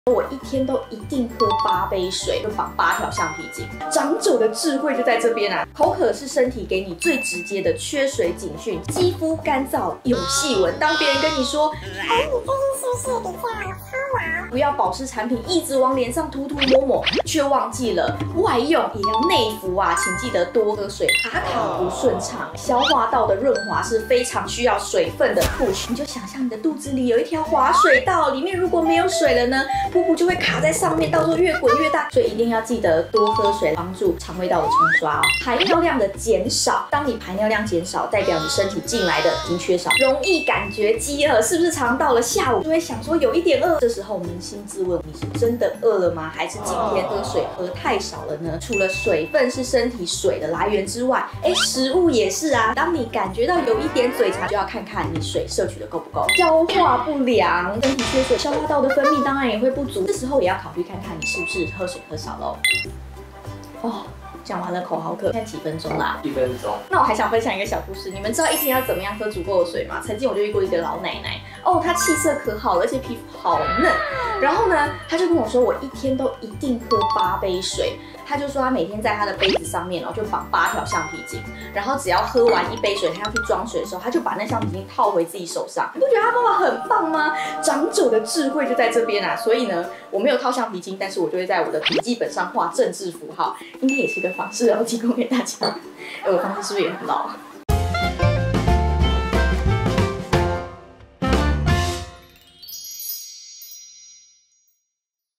口渴才喝水是錯的！營養師大推阿嬤的智慧｜ @nutritionistmin ｜觸健康
授權／ @nutritionistmin